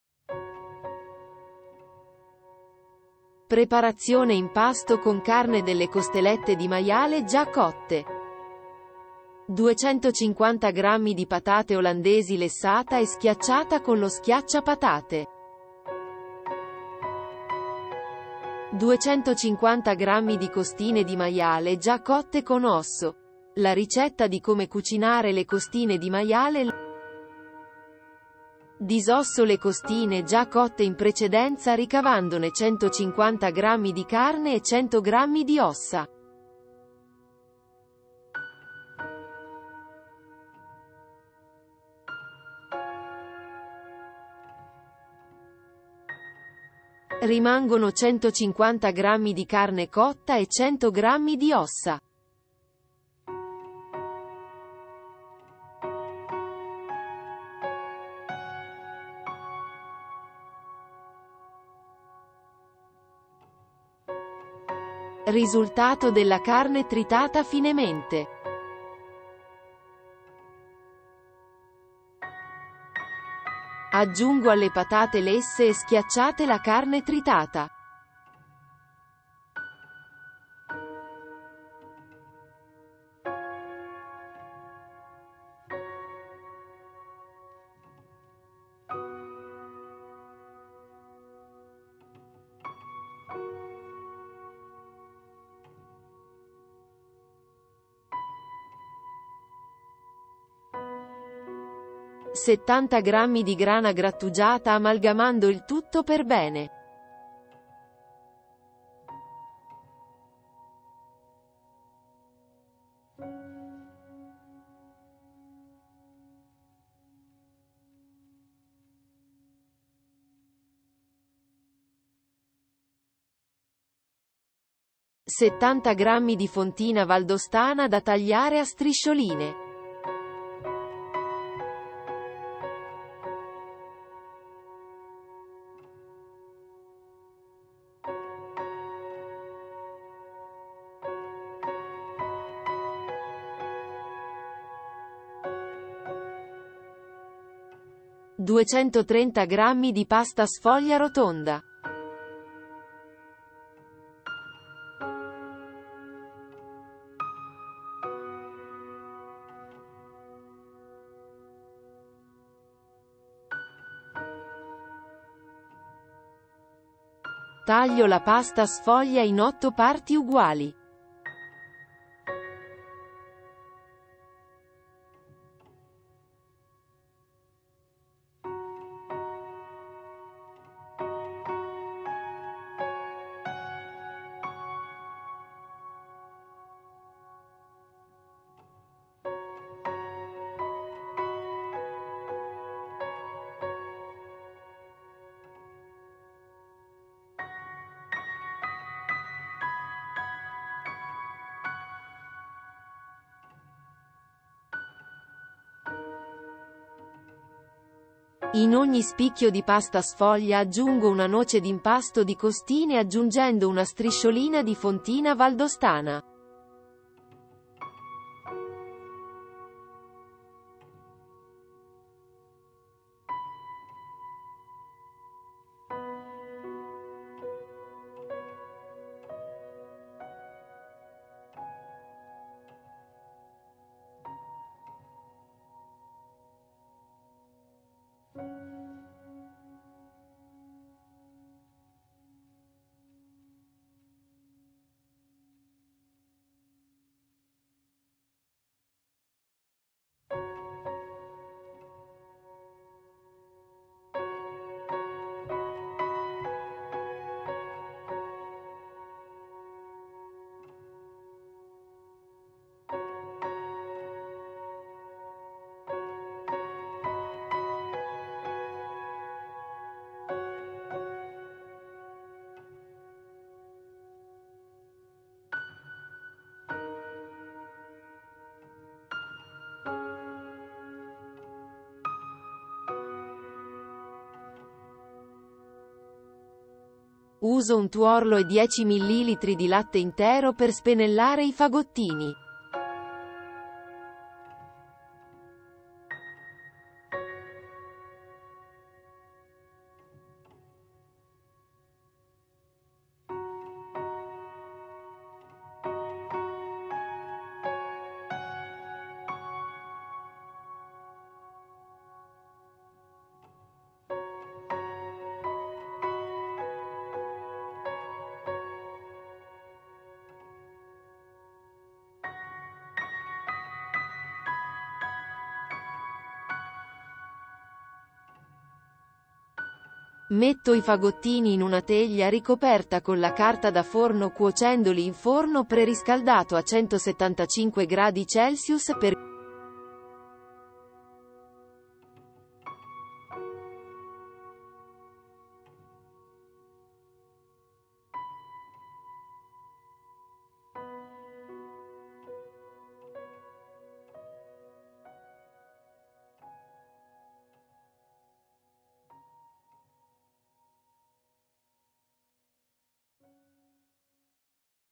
come fare i fagottini di pasta sfoglia ripieni di carne di costine di maiale al forno semplice
ingredienti:
250 gr patata olandese lessata
250 gr costine di maiale
70 gr di formaggio grana
70 gr fontina valdostana
1 tuorlo
10 ml di latte
230 gr pasta sfoglia rotonda